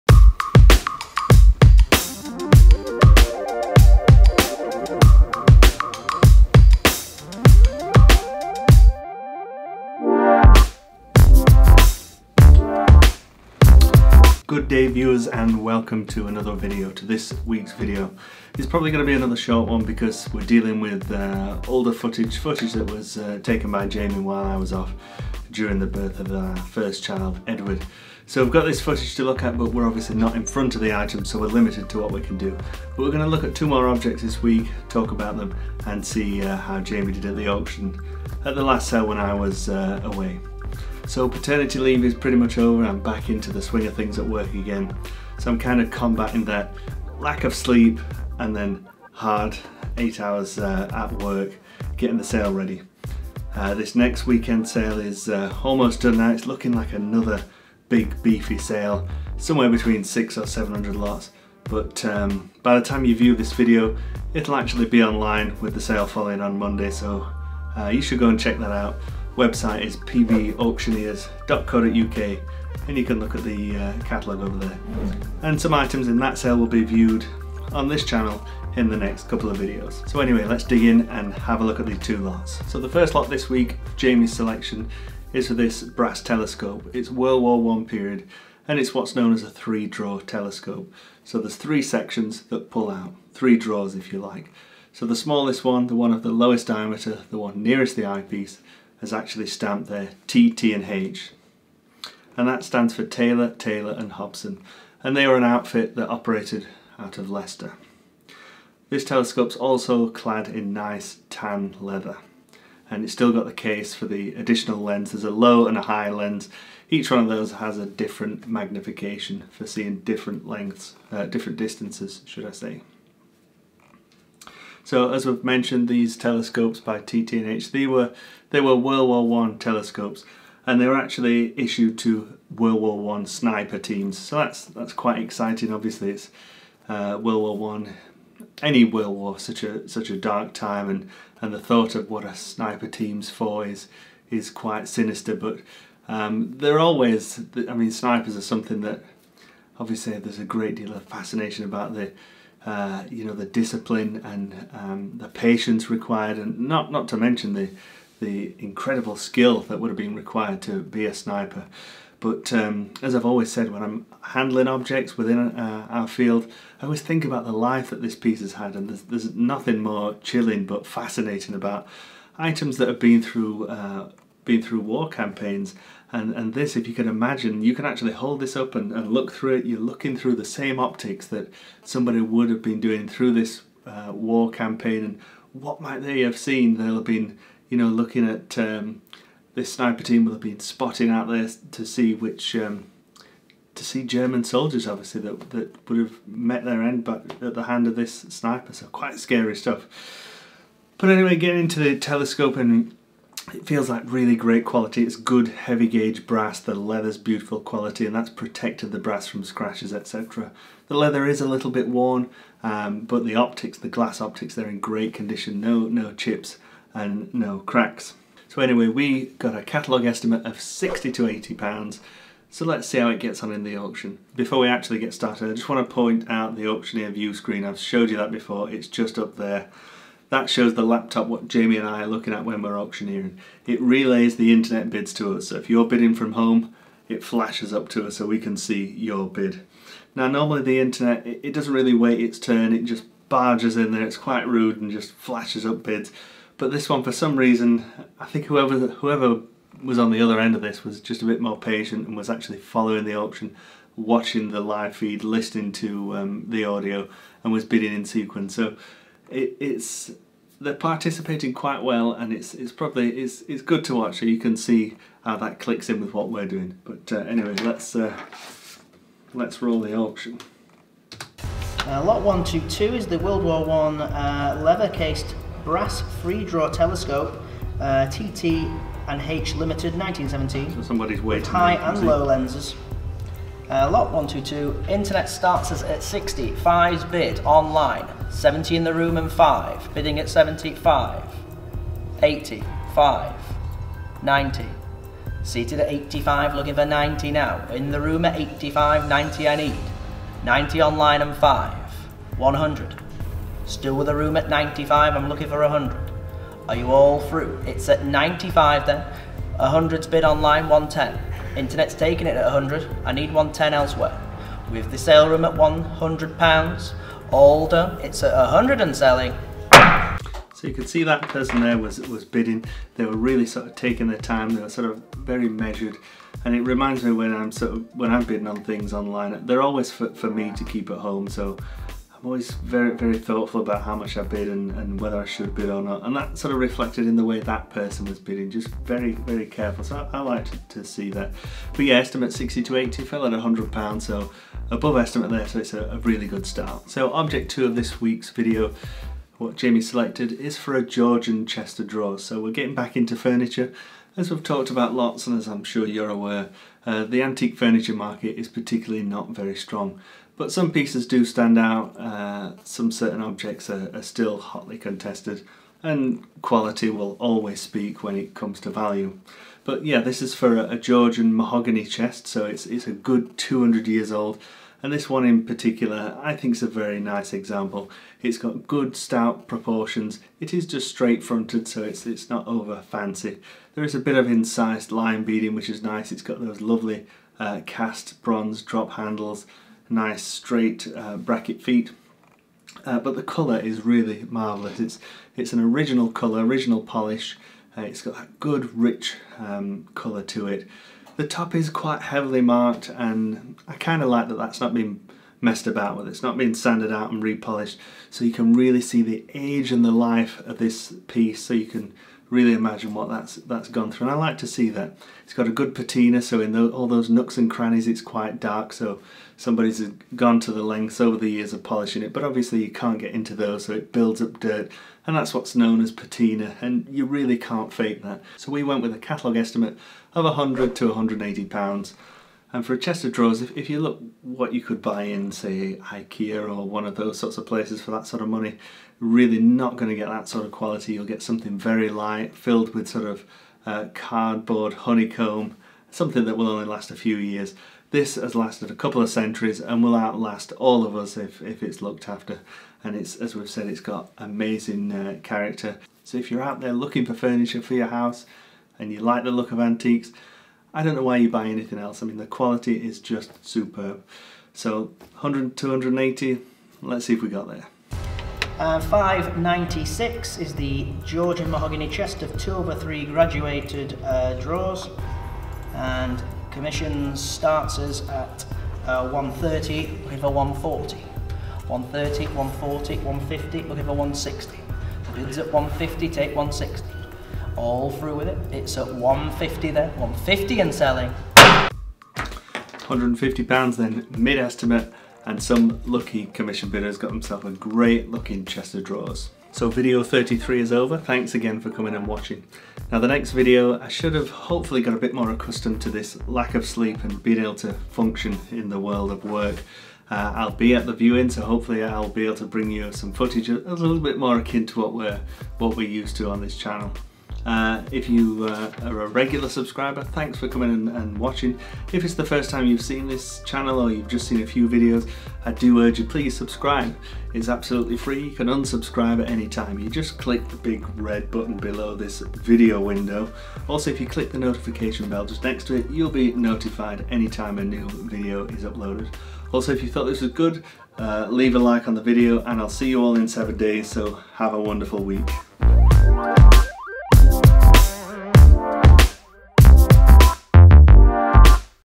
Antiques are for nothing!
More militaria and a Georgian mahogany chest of drawers go under the gavel.